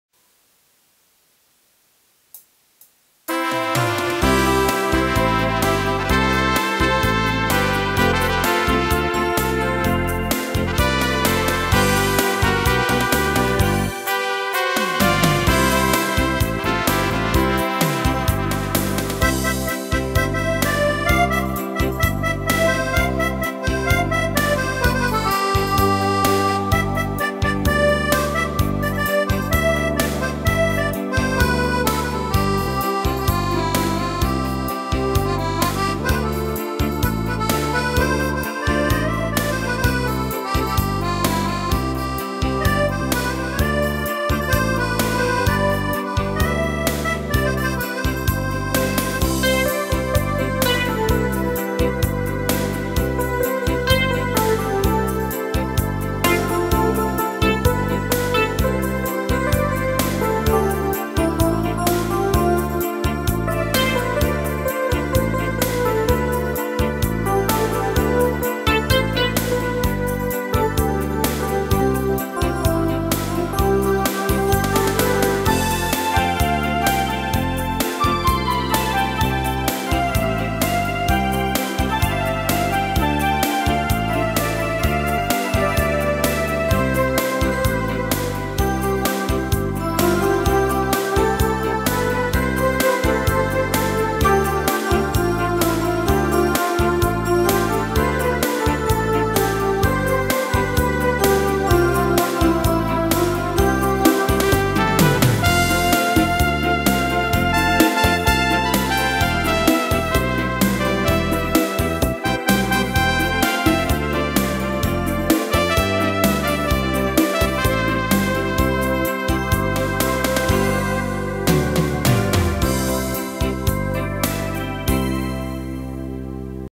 schlager
j'utilise le genos 2 je suis auteur de ma musique